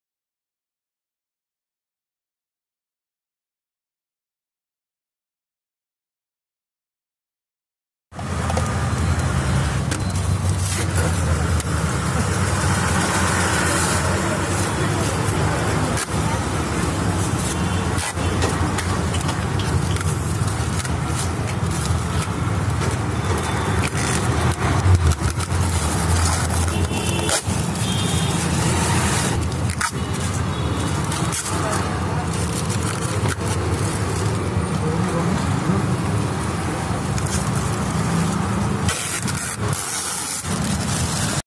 removing tainted black film from car window at SJROTARY Supervision of Mirchok Tr.C I Haseebullah
Good morning sir removing tainted black film from car window at SJROTARY under traffic Mirchowk with regards Delta17